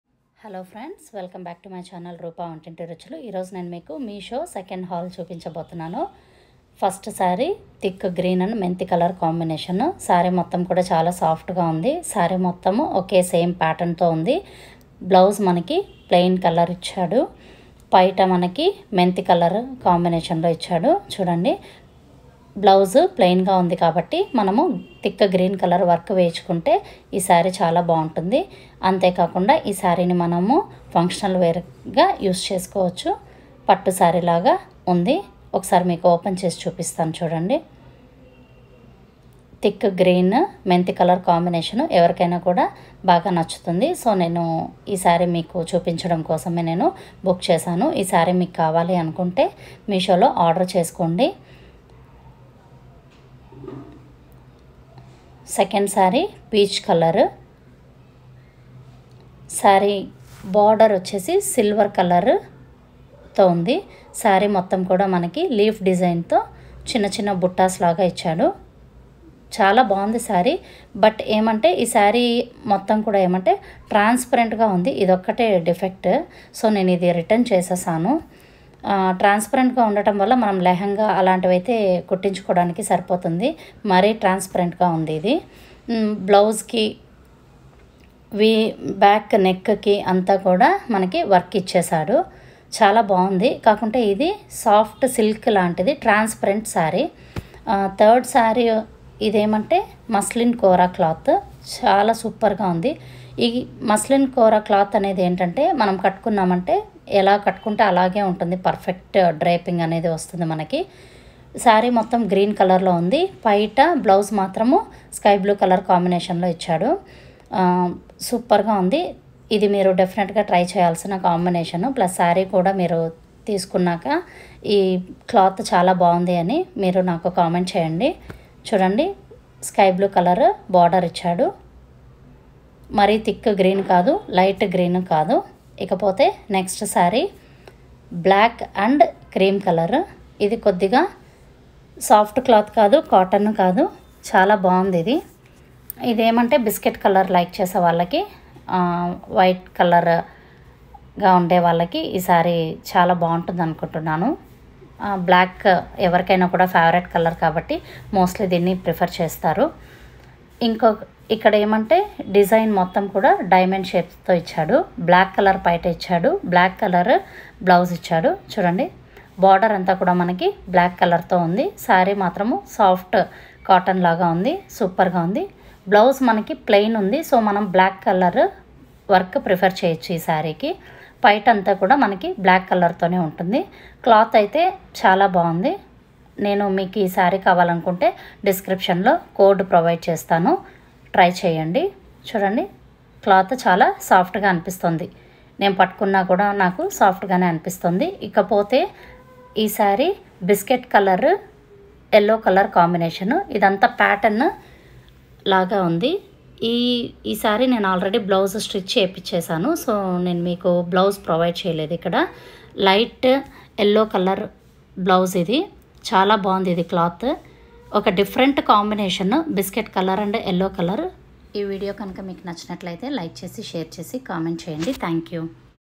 cheap and best sarees |Meesho saree haul 2 |with codes and details
1.thick green -20566056-cost 667/-
2.peach colour-62146231-cost-1107/-
3.parrot green-34831779-cost-738/-
4.black-30909672-cost-975/-
5.Biscuit colour-13053641-cost 999/-